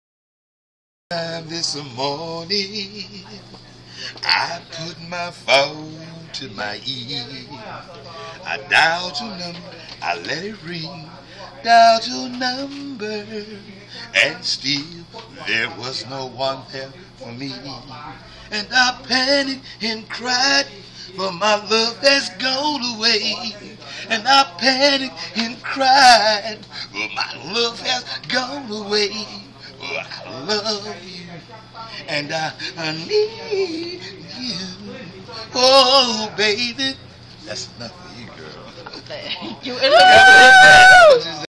Grandpa singing. Long Beach Citi Market
Grandpa Joe got voice hahha